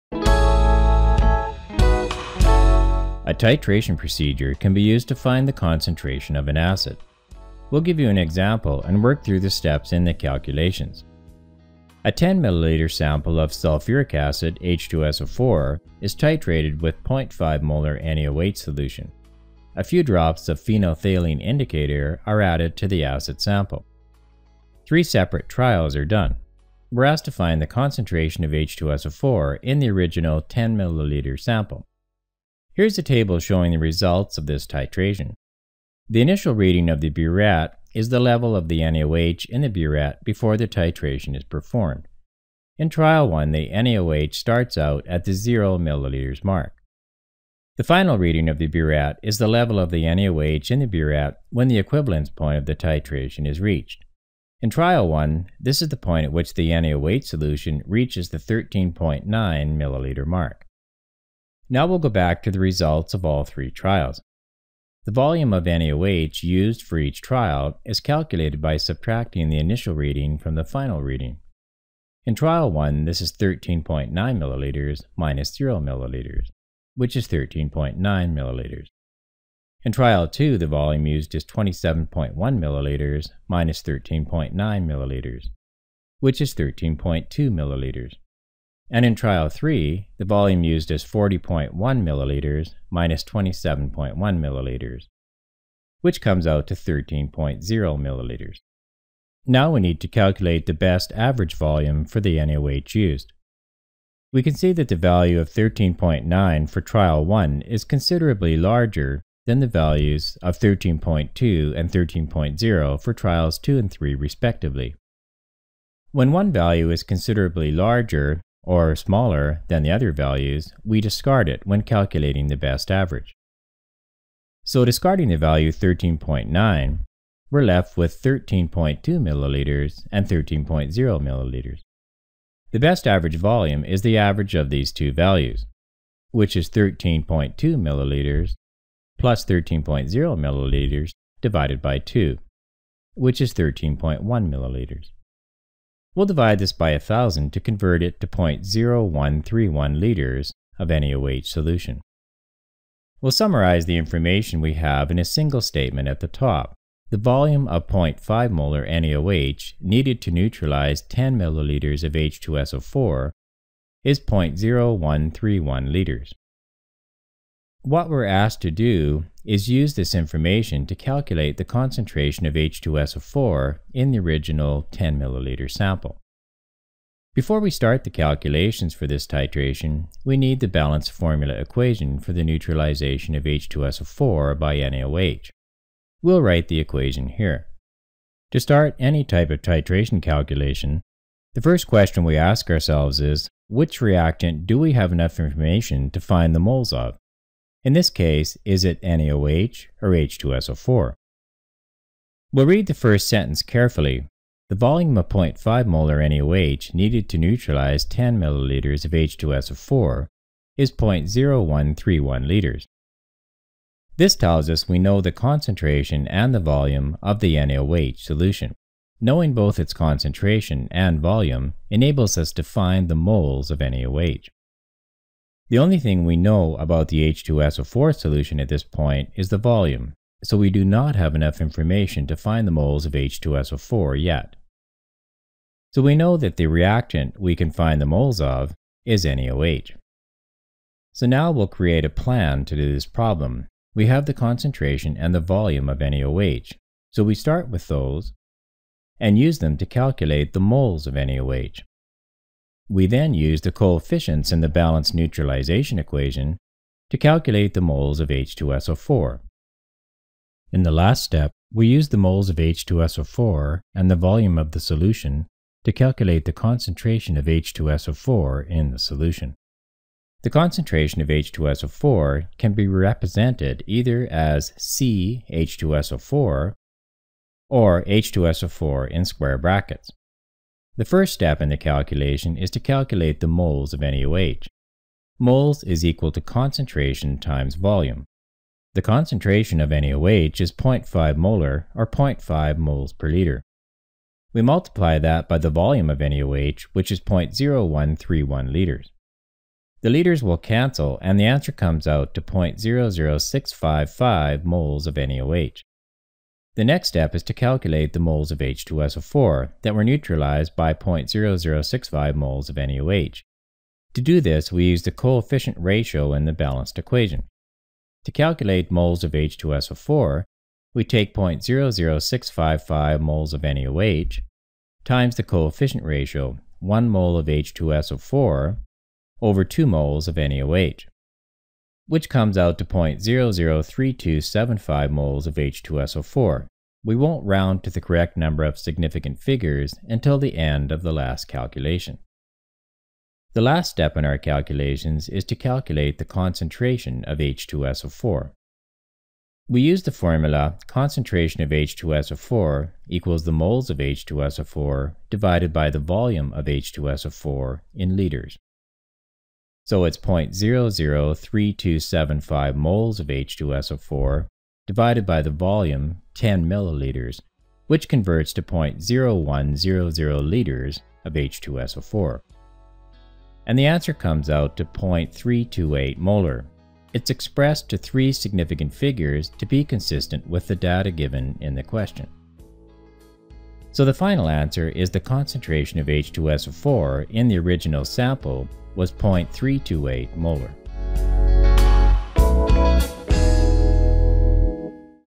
WCLN - Acid Base Titrations -- Part 1 - Chemistry
The concentration of an acid can be determined by titrating it with a base of known concentration. This video gives an example of such a titration using three separate trials, and it illustrates how the results are used to calculate the concentration of the acid

0:03I take creation procedure can be used to find the concentration of an acid
0:07will give you an example and work through the steps in the calculations
0:11a ten milliliter sample ups of uric acid
0:14h2so4 is take trade with .5 Moller Anne awaits addition
0:19a few drops a pheno failing indicator
0:22are added to the acid sample three separate trials are done
0:26rest to find the concentration of h2so4
0:29in the original ten milliliters sample
0:32here's a table showing results of this state region
0:36the initial reading at the beer at is the level up the NaOH
0:39in a beer at before the Tate region is performed in trial 1 a Neoh starts out
0:44at 20 milliliters mark
0:47the final reading at the beer at is the level up the any wage in a beer at
0:51when the equivalence point the tight region is reached in trial 1
0:55this is the point at which the any weight solution reaches the 13-point
0:59nine-millimeter mark
1:01now will go back to the results a ball three trials
1:04the volume up any wage used reach trial is calculated by subtracting the initial
1:09reading from the final reading
1:12in trial 1 misses 13.92 leaders
1:15-0 milliliters which is 13.99 leaders
1:19and try out to the volume used is twenty seven point one milliliters
1:23-13 point nine milliliters
1:26which is 13 point two milliliters
1:28and in trial 3 the volume used as forty point one milliliters
1:32minus twenty seven point one milliliters
1:35which comes out to 13.09 leaders
1:39now we need to calculate the best average volume for the any wat used
1:44can see that the value 13.99 for trial one
1:47is considerably larger in the values a 13-point to %um 13.04 trials 2&3
1:54respectively
1:56when one guy who is considerably larger or smaller
1:59than the other values we discarded when calculating the best average
2:04so discarding a value 13.99 left with thirteen point two milliliters
2:10and 13.08 milliliters
2:12did establish volume is the average up these two Ballys
2:16which is 13 point two milliliters
2:19plus 13.02 milliliters divided by two
2:23which is 13 point one milliliters
2:26will divide this by 8,000 to convert it to point 0131
2:29leaders have any weight solution
2:33will summarize the information we have been a single statement at the top
2:36the volume a .5 molar Neoh needed to neutralize ten milliliters h2so4
2:44is point 0131 leaders
2:47what were asked to do is use this information to calculate the
2:51concentration of h2so4
2:53in the original ten milliliters sample
2:57before we start the calculations for the staycation we need to balance formula
3:01equation for the neutralization a beach to SF for a biennial wage
3:06will write the equation here
3:08to start any type a tight range in calculation the first question we ask
3:12ourselves is
3:13which reacting do we have enough information to find the malls are
3:17in this case is it Neoh her h2so4
3:22worried the first sentence carefully the following the .5 mall there any wage
3:26needed to neutralize ten milliliters to be h2so4
3:30his point 0131 leaders
3:34this does this mean oh the concentration and the volume
3:37up the NaOH solution knowing both its concentration
3:40and volume enables us to find the malls have any wage
3:45the only thing we know about the h2so4 solution at this point is the volume
3:50so we do not have enough information to find the malls and h2so4 yet
3:55so we know that the reactant we can find the malls are is any wage
4:01so now will create a plan to his problem we have the concentration and the volume
4:05a penny a wage
4:06so we start with those
4:09and use them to calculate the malls are many wage
4:12we then use the coefficients in the balance neutralization equation
4:17to calculate the malls and h2so4
4:20in the last step we use the malls h2so4
4:23and the volume up the solution to calculate the concentration of h2so4
4:28in the solution
4:30the concentration of h2so4 can be represented
4:33either as see h2so4
4:37or h2so4 in square brackets
4:40the first step in the calculation is to calculate the malls have any weight
4:45malls is equal to concentration times volume
4:48the concentration have any wages .5 molar
4:51or .5 miles per year
4:54we multiply that by the volume a venue age which is point 0131 leaders
4:59the leaders will cancel and the answer comes out 2.00 655 moles of NaOH
5:06the next step is to calculate the mall zip h2so4
5:09they were neutralized by point 0065 malls have any wage
5:14to do this we used to call fishing ratio and the balanced equation